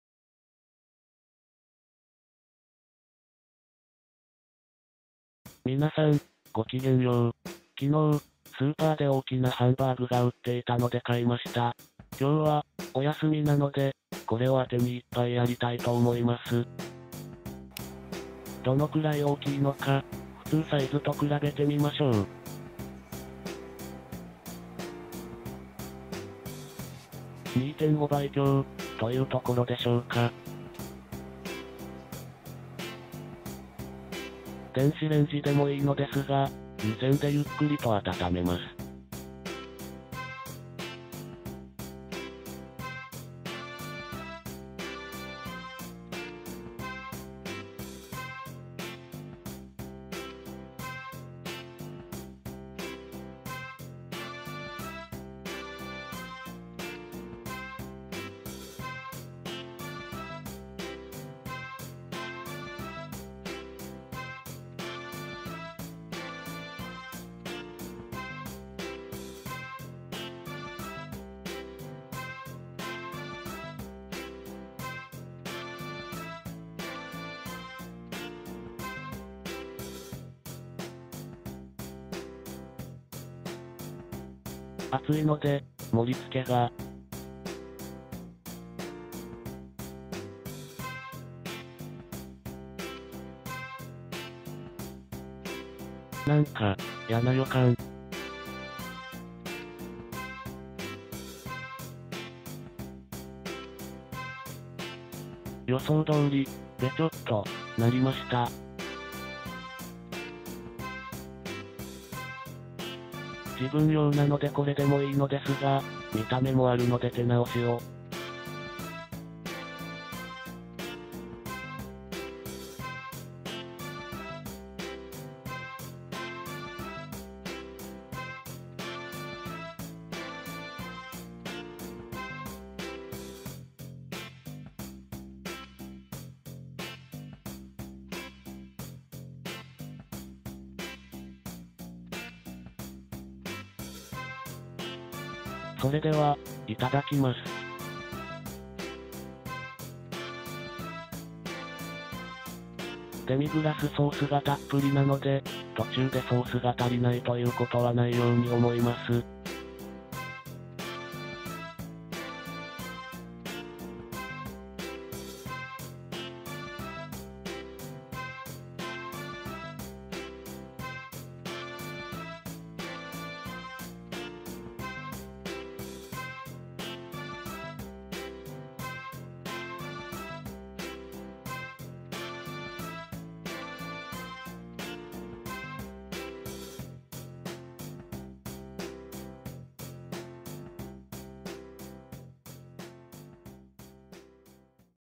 でかいハンバーグで昼酒
★音量が小さいと思われます。すいませんが調整してください。
福留ハムのハンバーグです。　物珍しさから買ってしまいました。